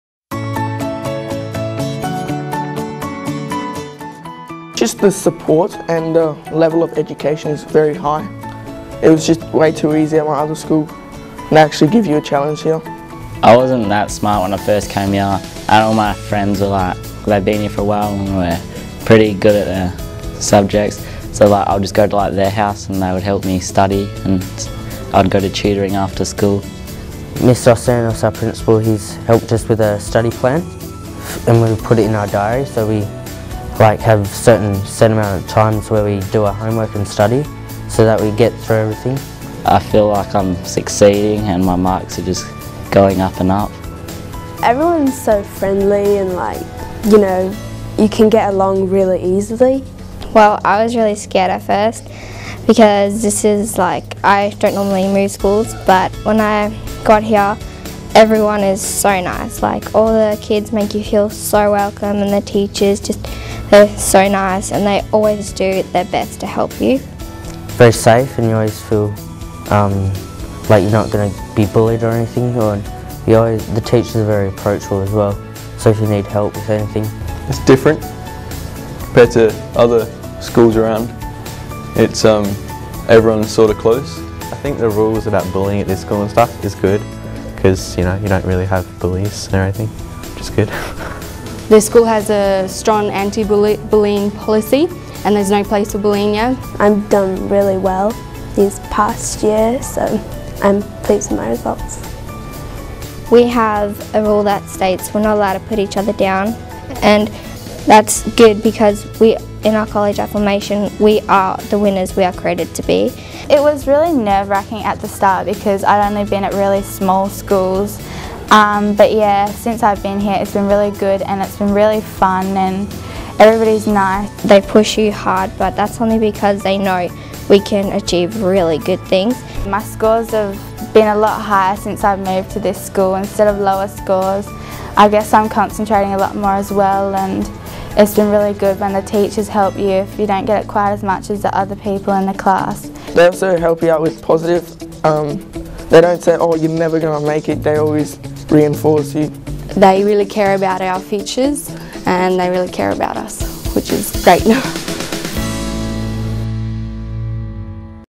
Austin Cove Baptist College Students Feedback
Students give feedback on their experience of Austin Cove Baptist College.